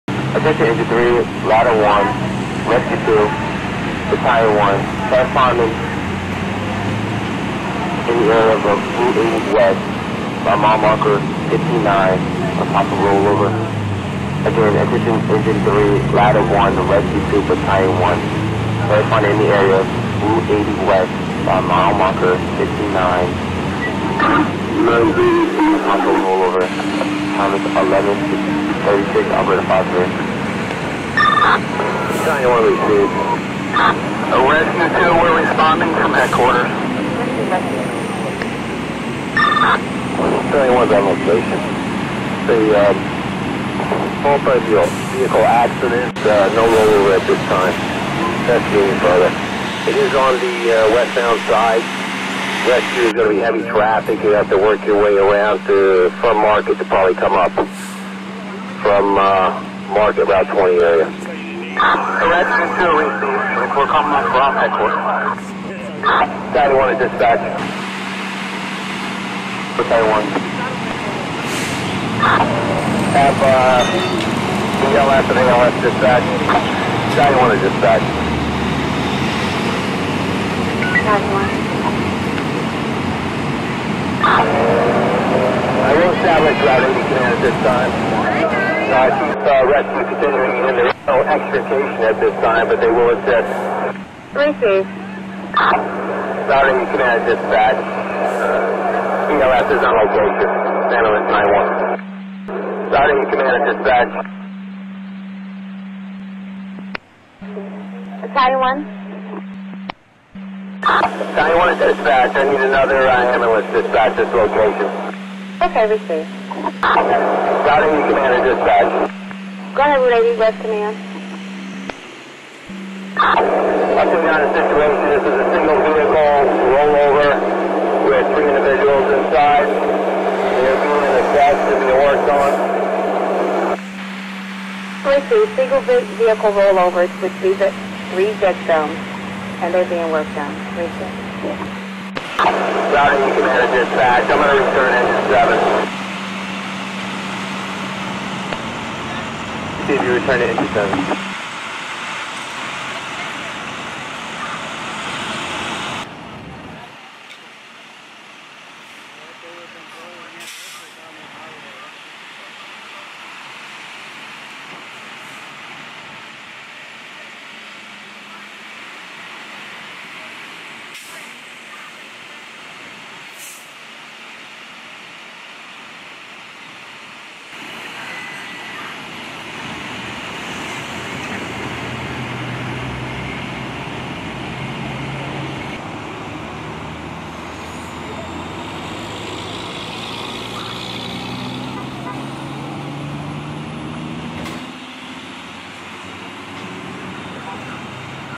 Paterson NJ Fire Dept operates at an Accident Interstate 80 WEST Multiple Injuries.
Paterson NJ Fire Dept operates at an Accident Possible Rollover   Interstate 80 WEST Multiple Injuries.